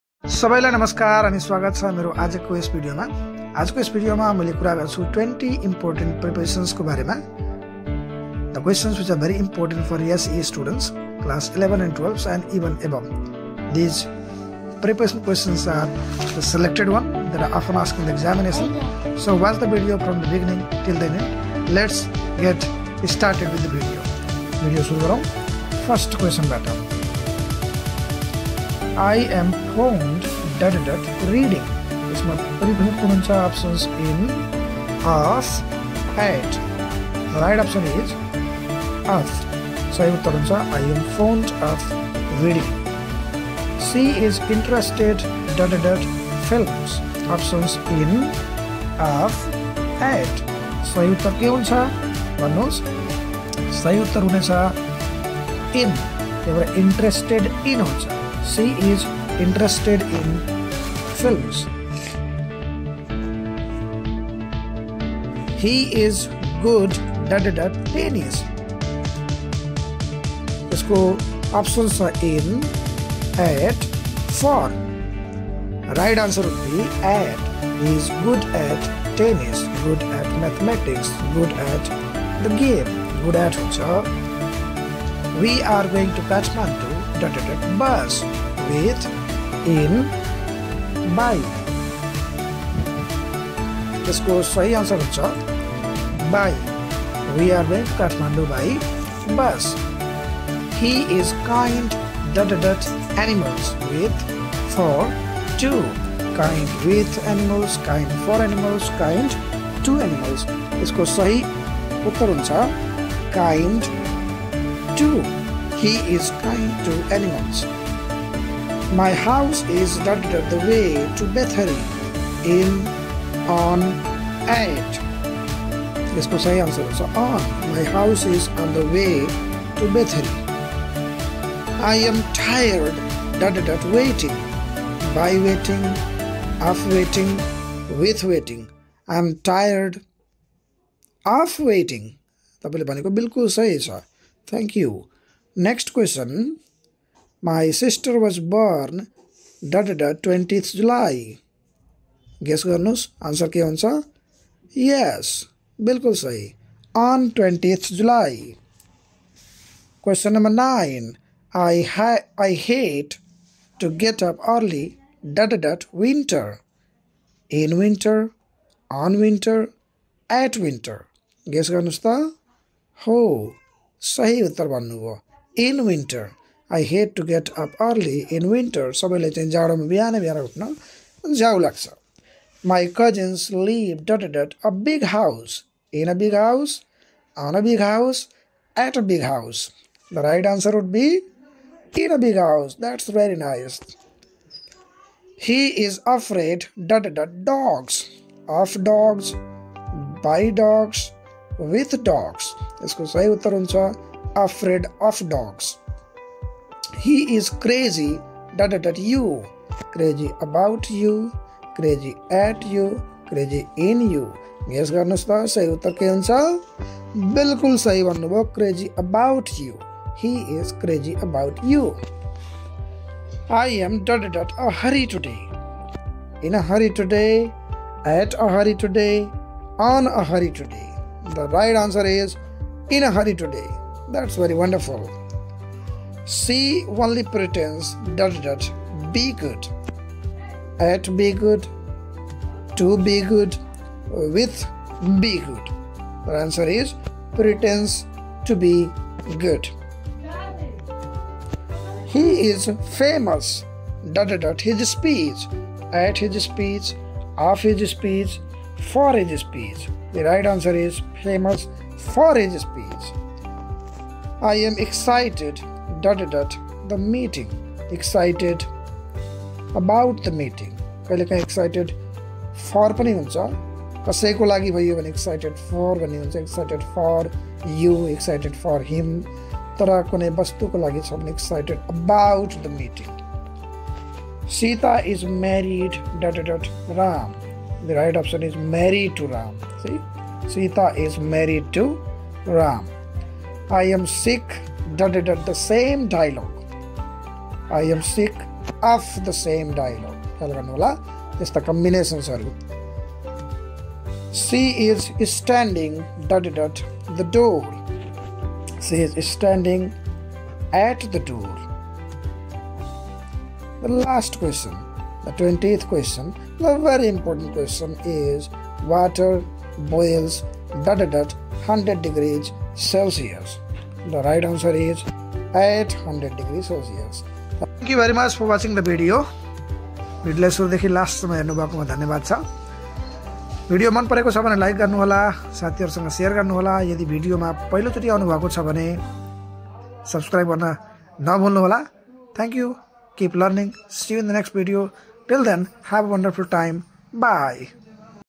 Prepositions:20 Important Questions with Answers||SEE
Prepositions: 20 Important Questions with Answers. This video is set of 20 important preposition questions with answers. It is very useful for class 10 students and above. Watch the  video from the beginning till the end.